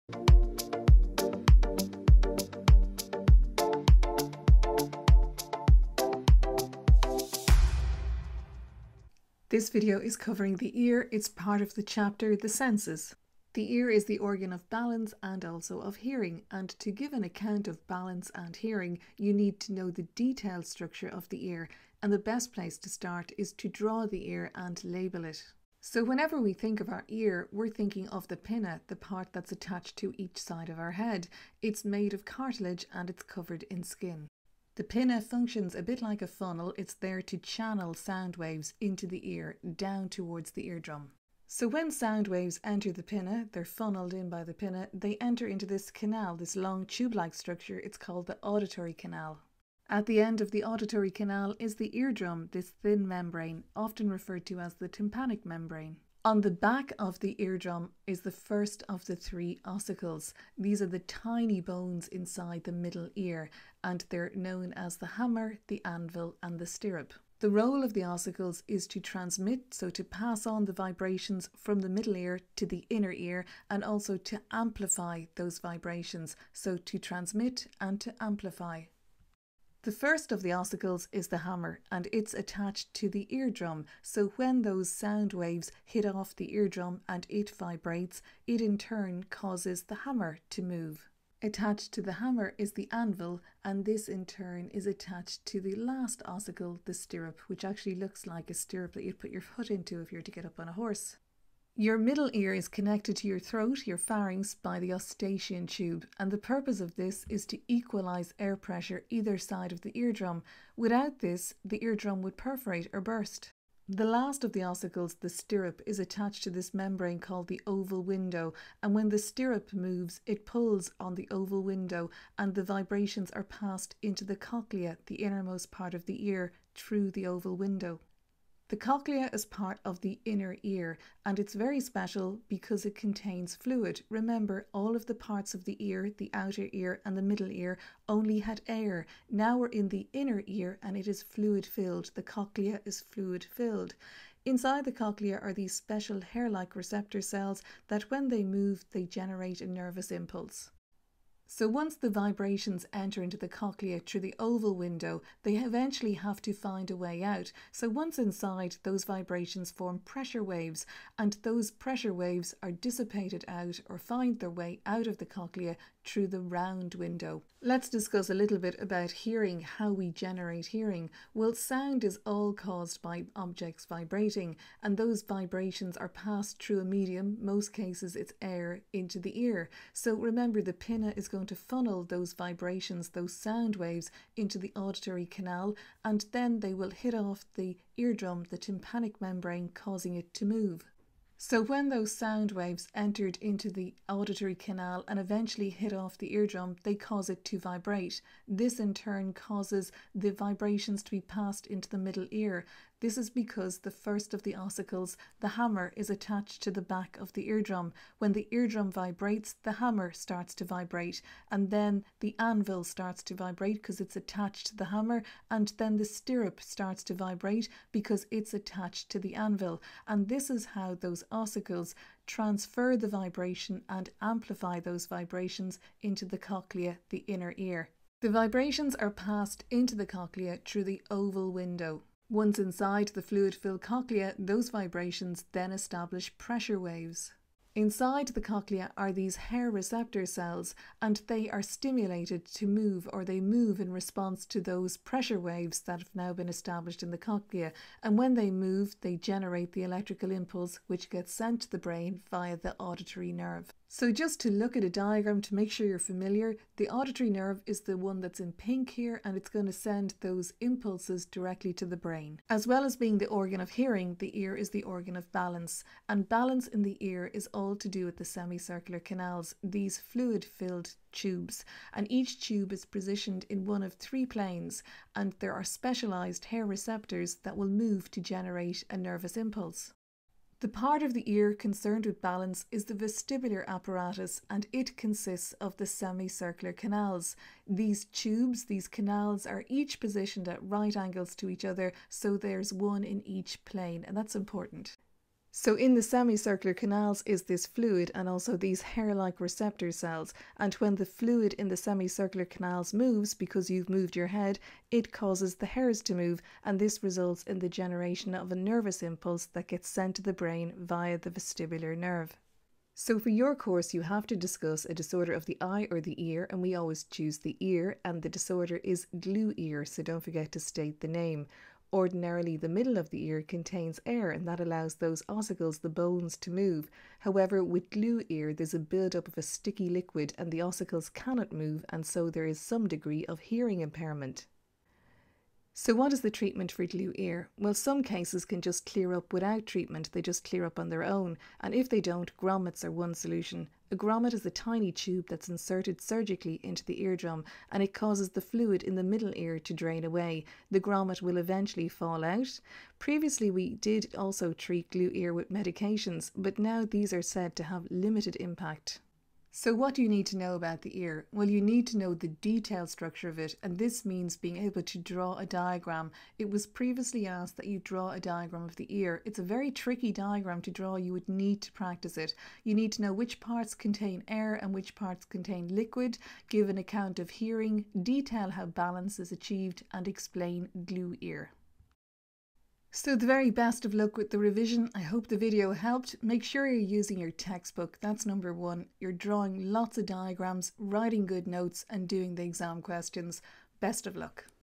The Ear (Updated)-Leaving Cert Biology
I updated this video to include the most current treatment for Glue Ear-medications that were previously thought to be effective are now not considered so. Just state Grommets in your exam. Also wanted to add a caption to state that the vestibular and auditory nerves are two branches of the Vestibulocochlear nerve ( don't need to know this-I just wanted it added).
This video is made to help my students study the ear and complete exam questions. It does not replace any textbook use.  The video covers the structure of the ear and balance in very basic detail. The only way to improve is to do past exam questions with the book open.